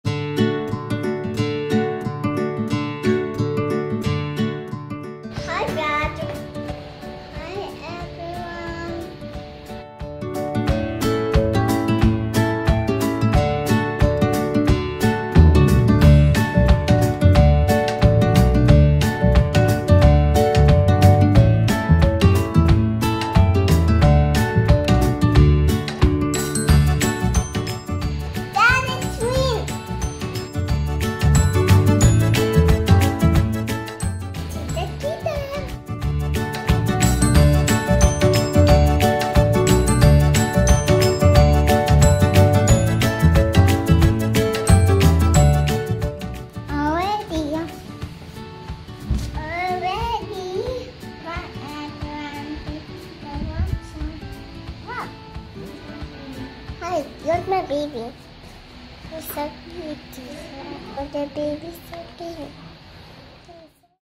Self Care Activity
Summer learns how to take care of herself. She brushes her hair, keeps her hands clean, and finally puts on socks and shoes. All part of growing up and learning to be independent.